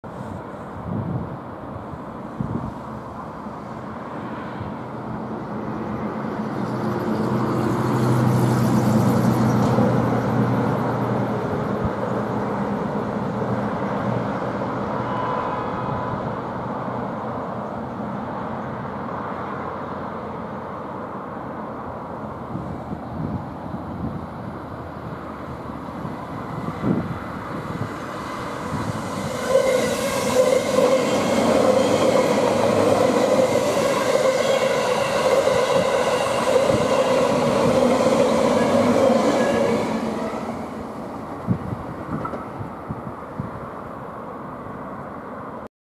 783系(8両) 特急ハウステンボス15号 ハウステンボス行(6015H)・みどり15号 佐世保行(4015M) けやき台駅通過
特急ハウステンボス15号 6015H(運転日注意)　博多14:32→ハウステンボス16:24　※早岐～ハウステンボス間ワンマン列車

特急みどり15号 4015M(毎日運転)　博多14:32→佐世保16:25

※2017年(土曜・休日)ダイヤ